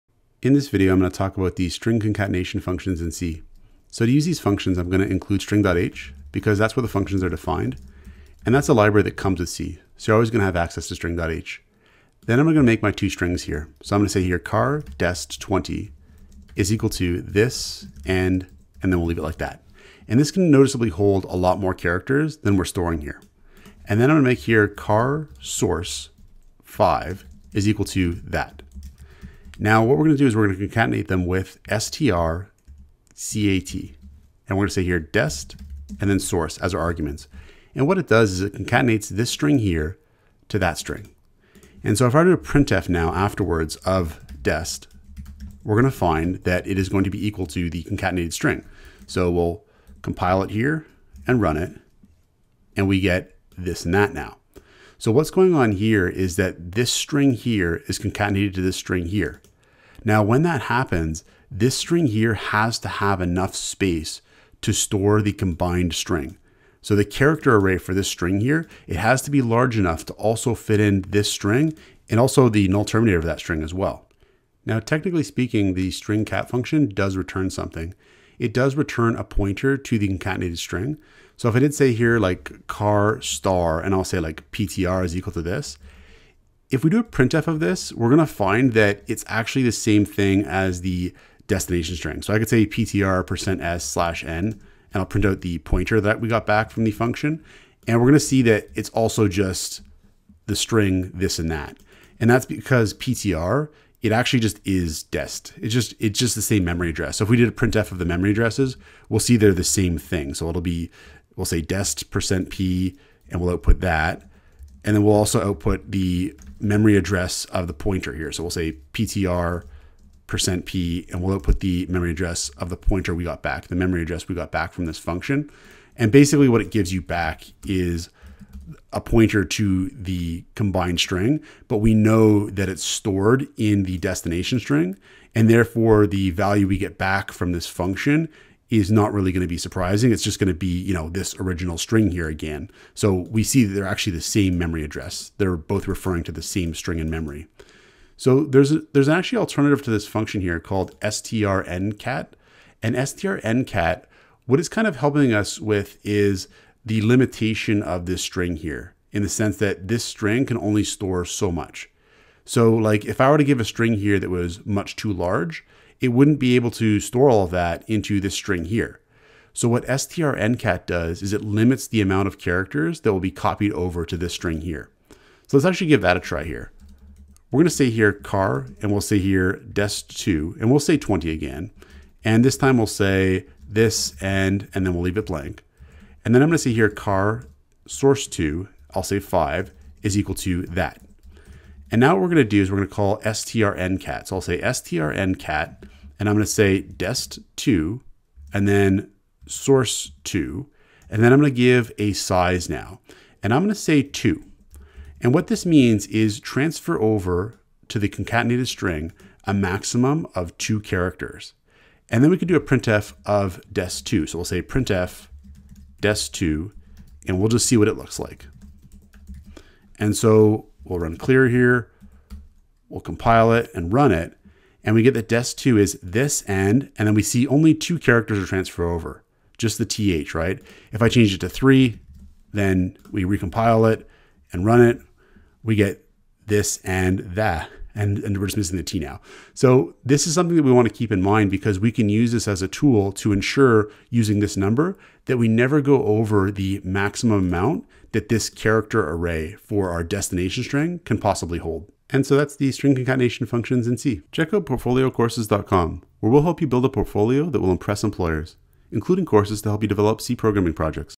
strcat() and strncat() functions | C Programming Tutorial
An overview of how to use strcat and strncat functions in C. Source code to build a portfolio that will impress employers!